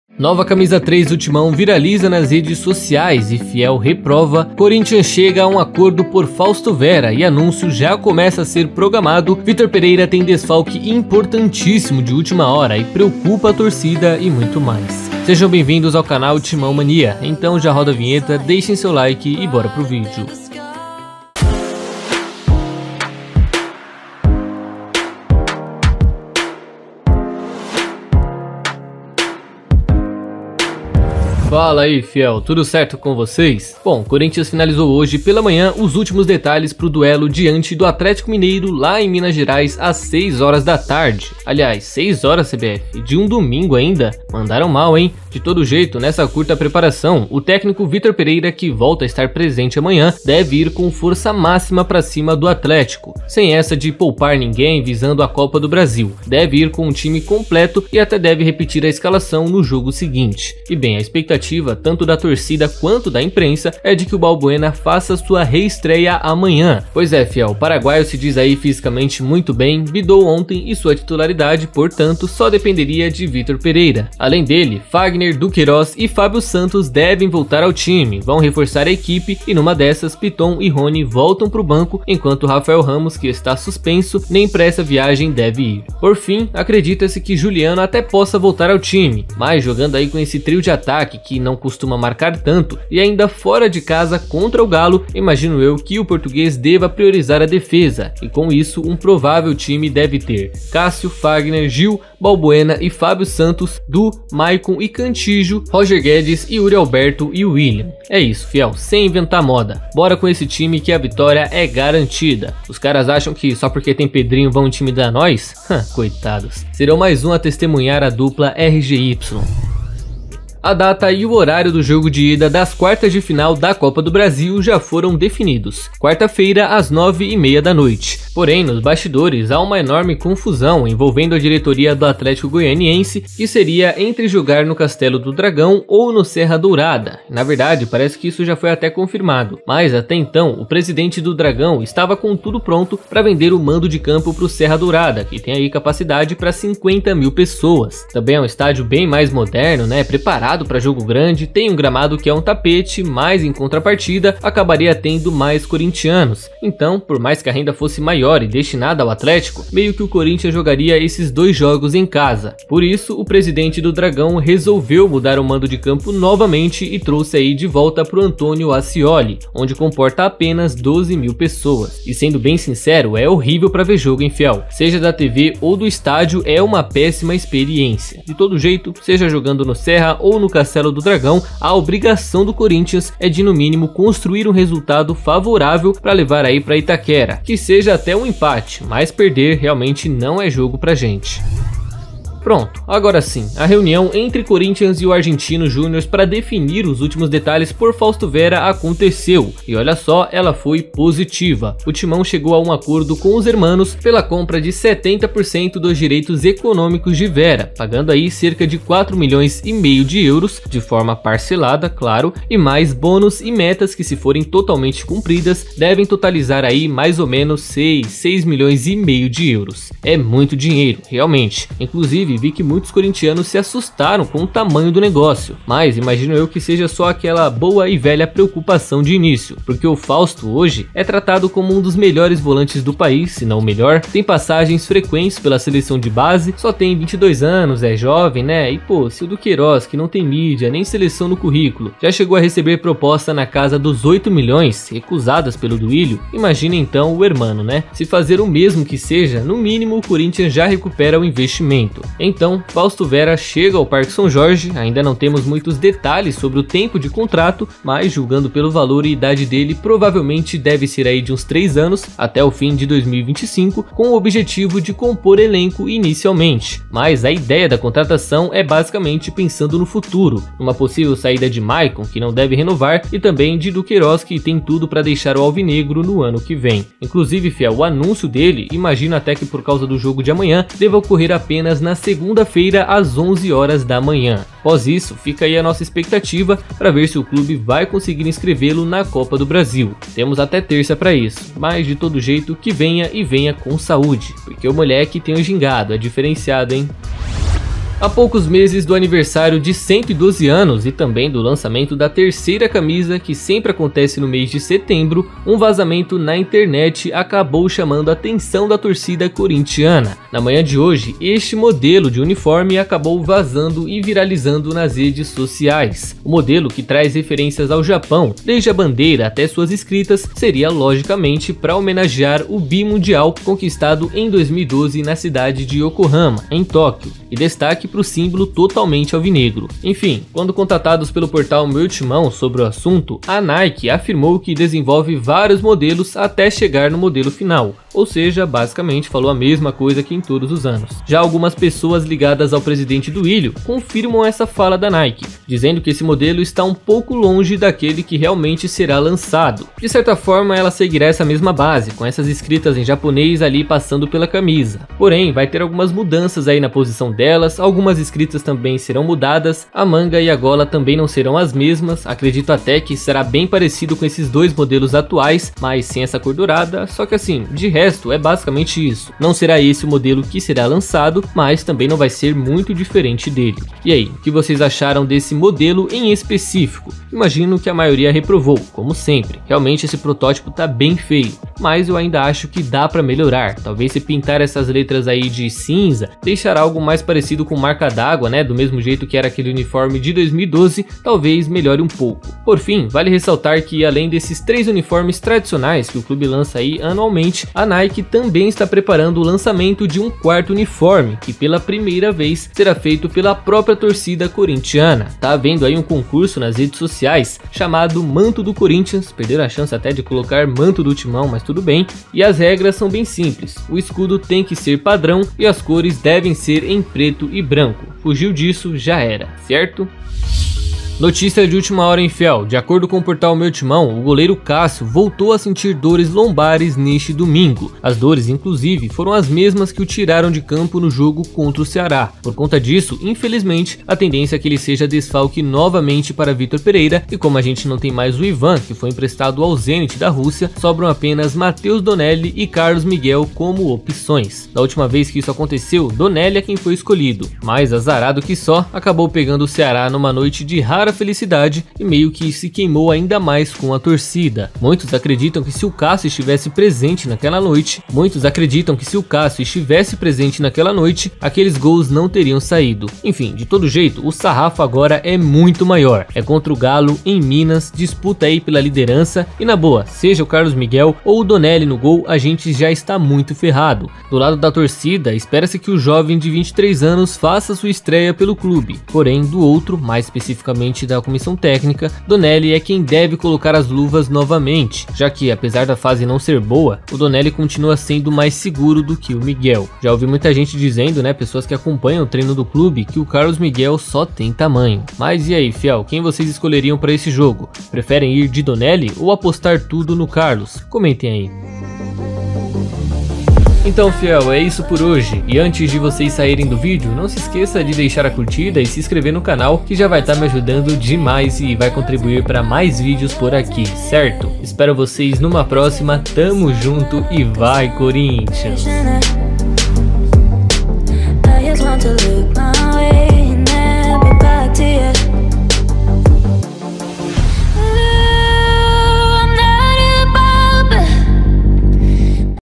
VAZOU NO TIMÃO! NOVIDADE CHOCA FIEL! | ACORDO POR F. VERA É ANUNCIADO | CÁSSIO VIRA DESFALQUE e+
▶️ COMPARTILHE COM A FIEL!